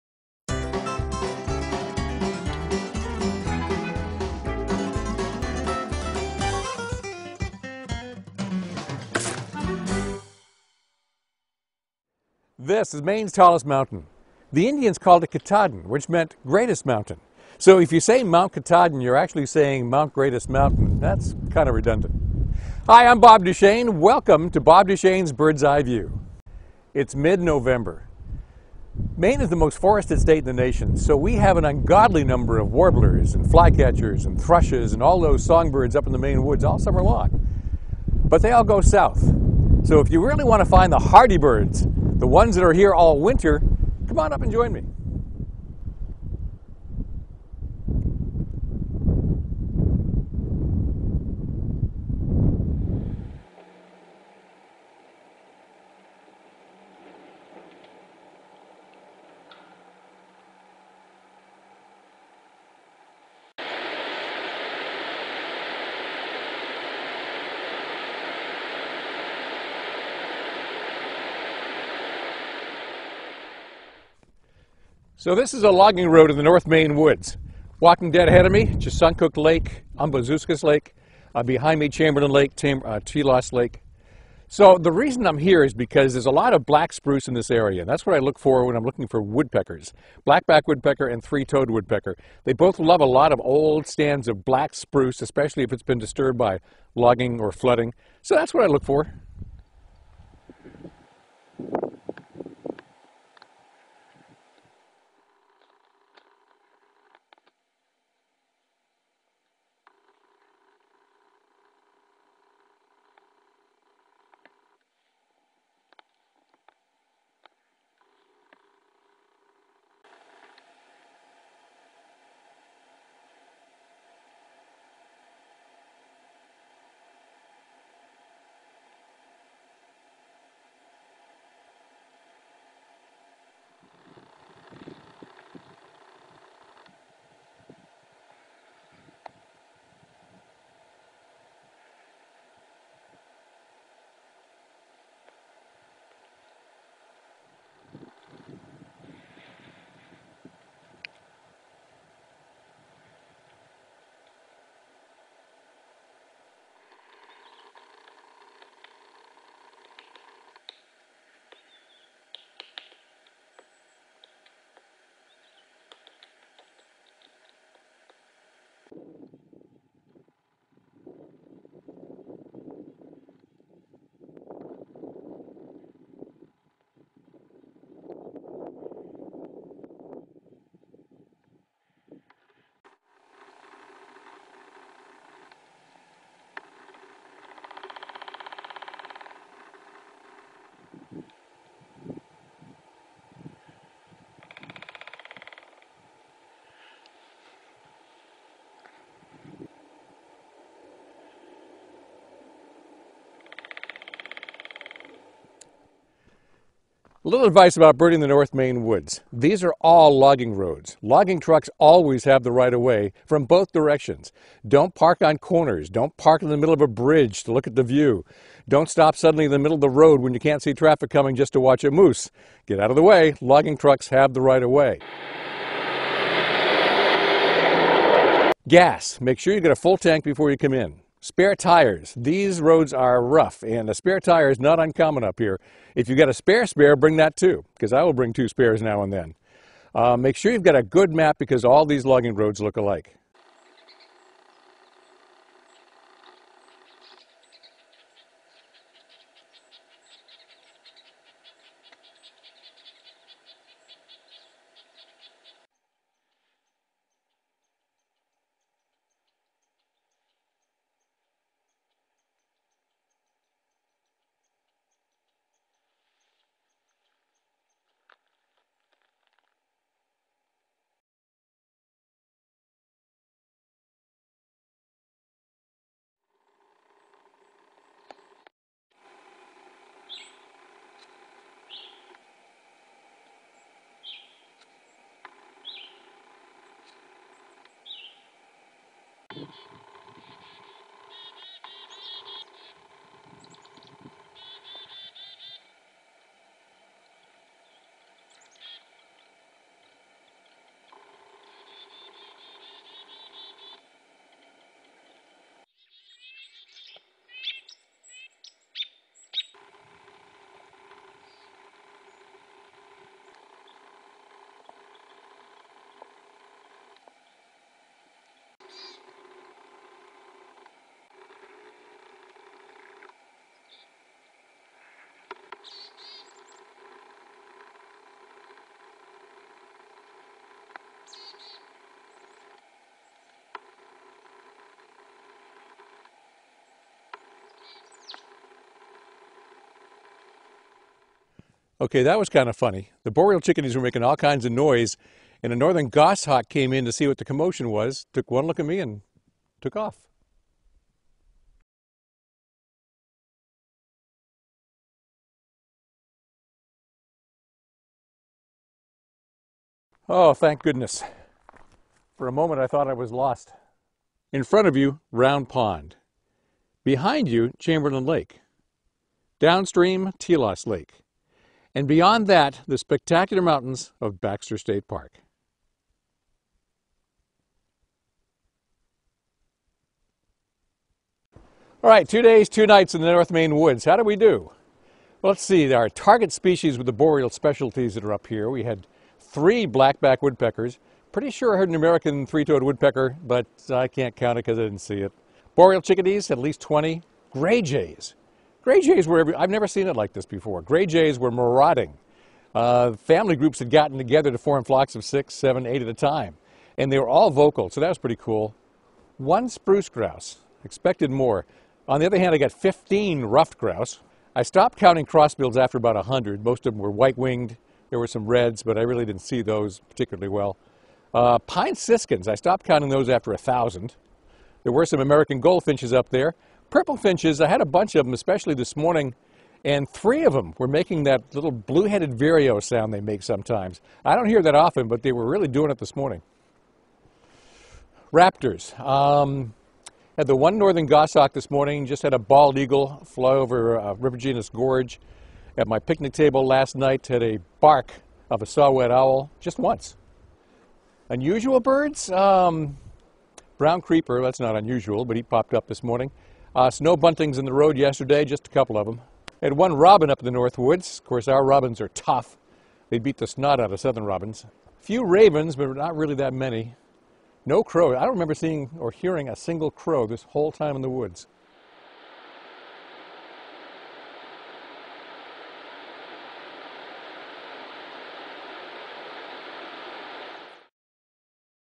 Boreal Birding in Winter
Birding the North Maine Woods in winter is special. Special birds of the northern forest, such as Canada jays, boreal chickadees, spruce grouse, black-backed woodpecker, and American three-toed woodpecker are findable. Tons of Canadian finches irrupt southward into Maine in the cold months. Awesome.